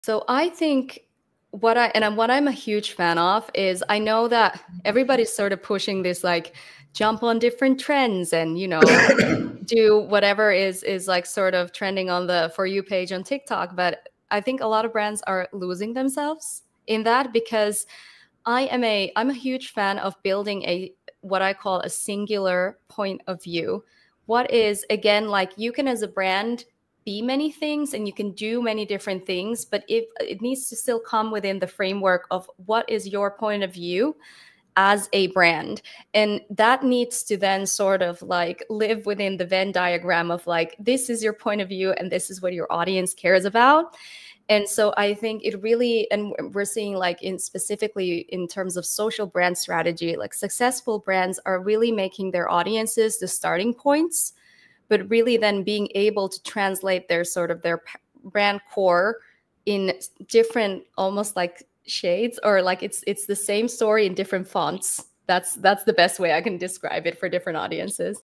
Crafting Powerful Brand Stories: Your Unique Point of View
Discover the secrets to creating compelling brand stories that resonate with your audience. Learn how to blend your brand’s perspective with audience interests for authentic engagement and lasting impact. Elevate your social brand strategy today!